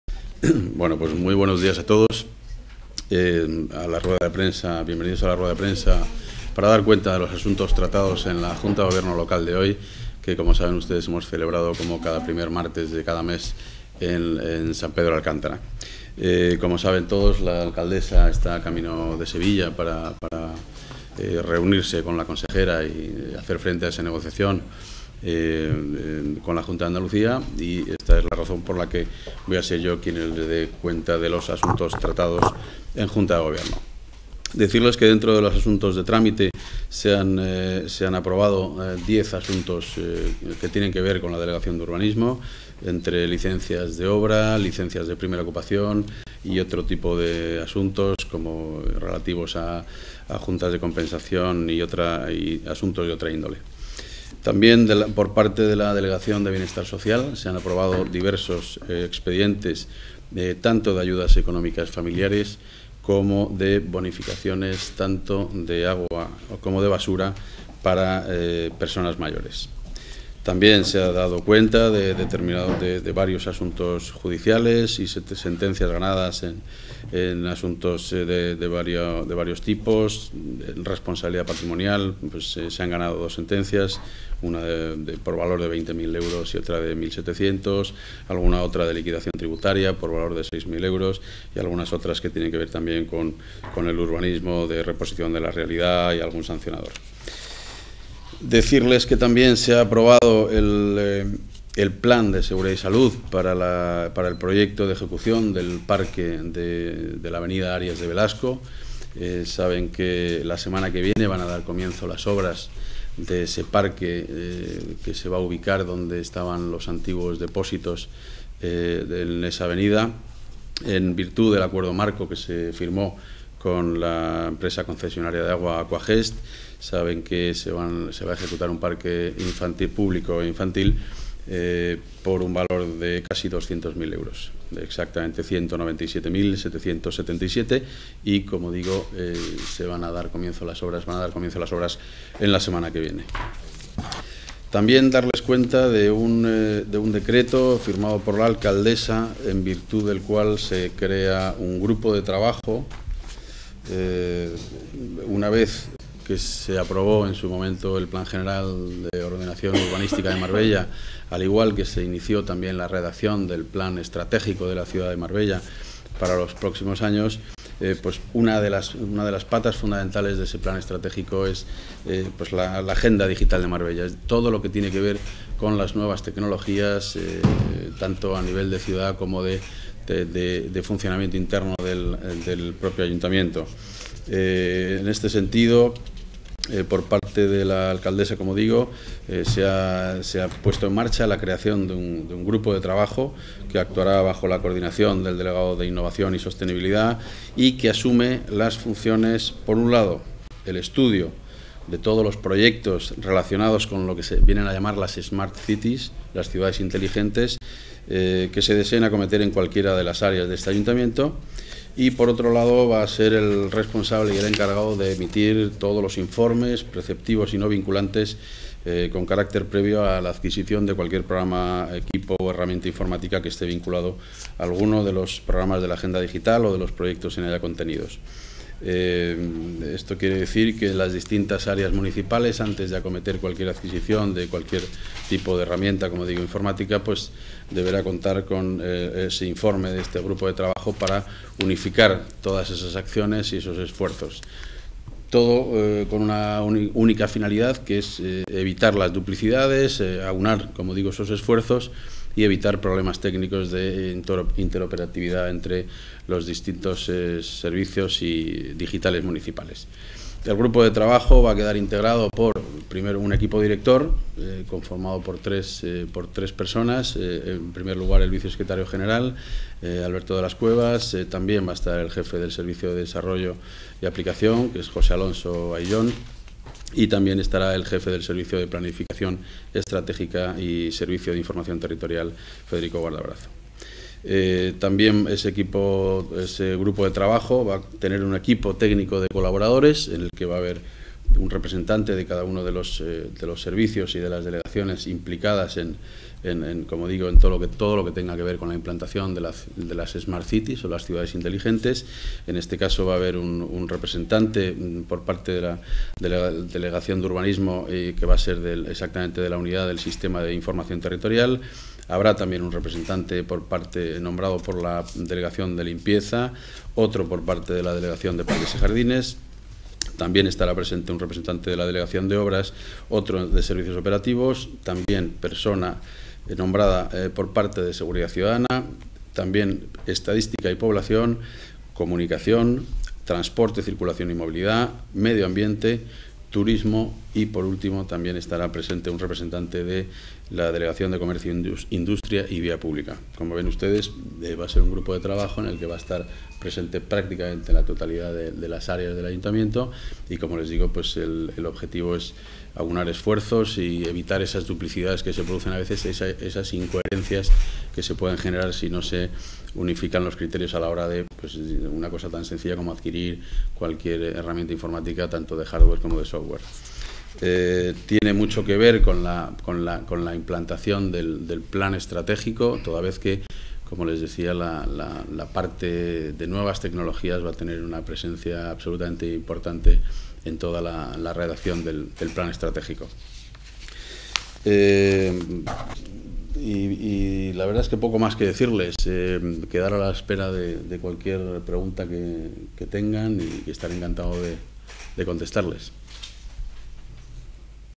Junta de Gobierno Local en San Pedro Alcántara, declaraciones de Pablo Moro. Acera de la Medina
El concejal de urbanismo del Ayuntamiento de Marbella contesta a la  pregunta de una periodista sobre el  local de Acera de la Marina (Marbella)